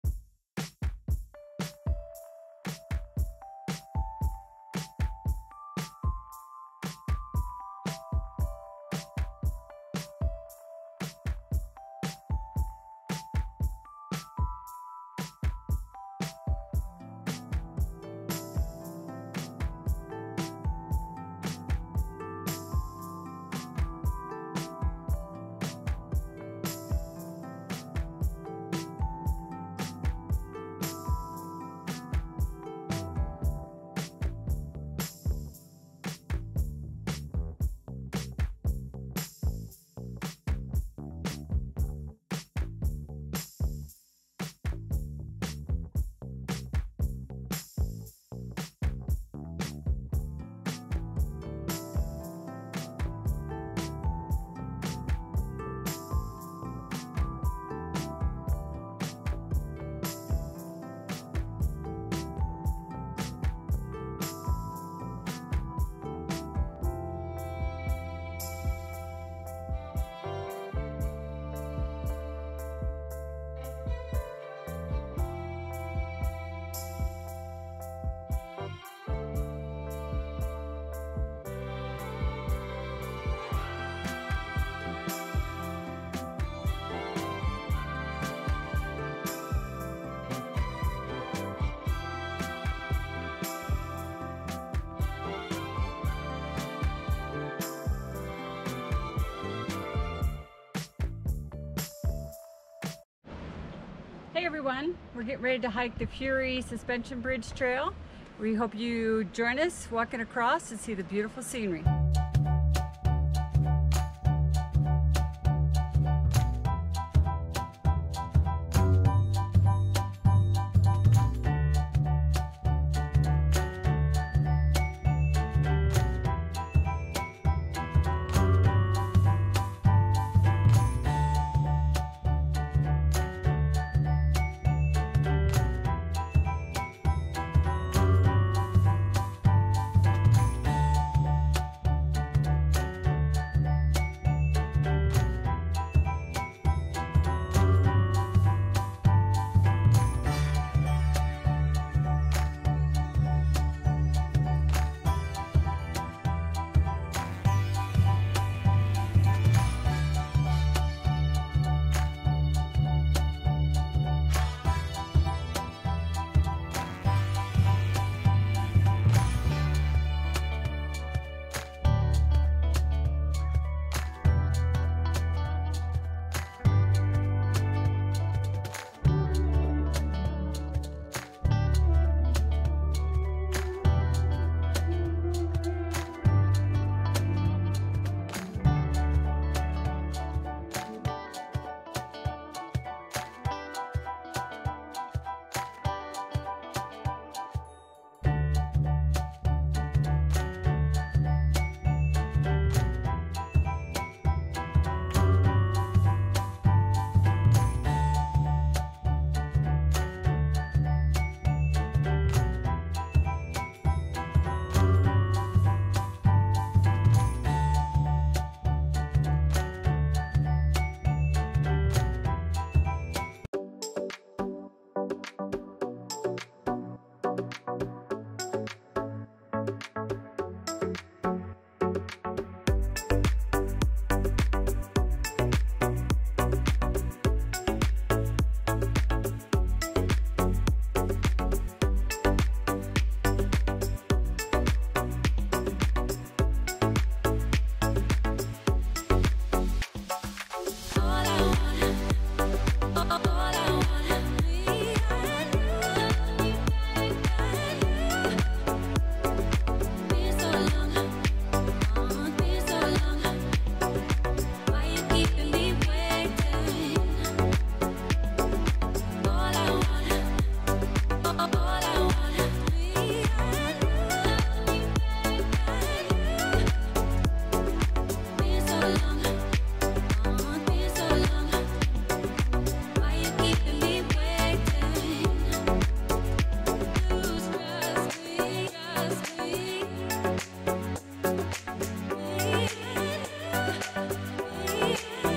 Surviving the Furi Suspension Bridge!
Our video shares directions and scenery as we walk from the Furi gondola station to the Furi Suspension Bridge